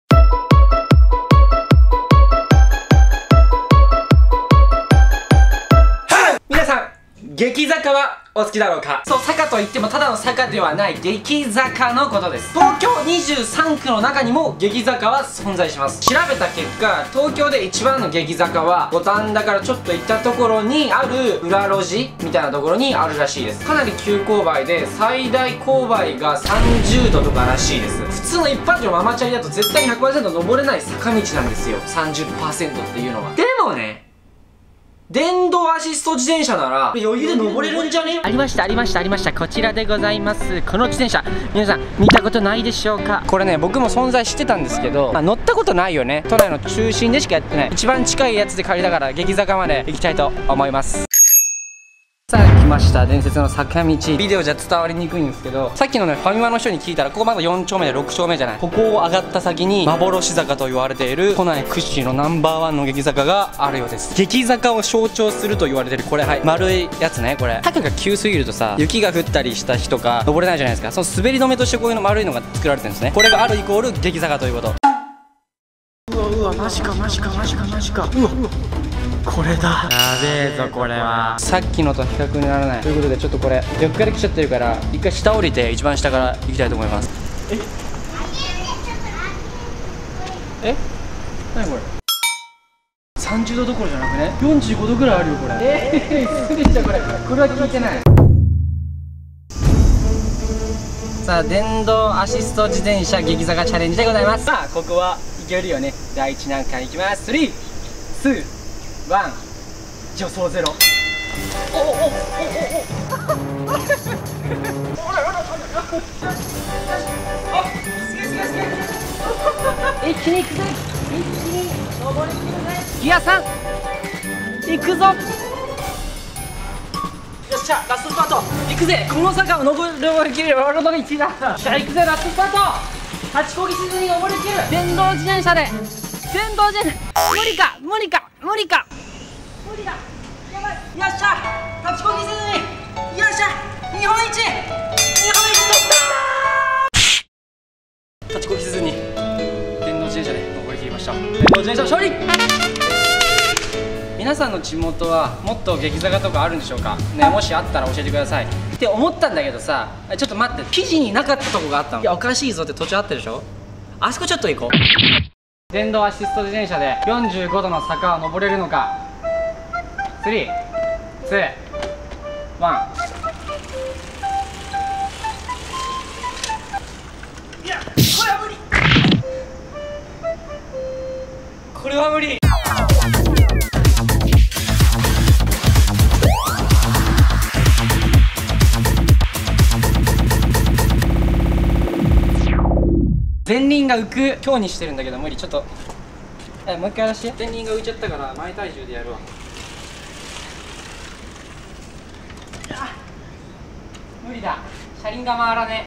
【激坂チャレンジ】ありえないほど急な坂を電動自転車で登る!!!!!
都内一の激坂がある品川区北品川6丁目の坂で激坂チャレンジしてきましたw流石に素人が普通にやるのは無理だから電動アシスト自転車で･･･

ひろちゃんねるでは面白動画、インテリア動画、パズドラ実況をアップしています。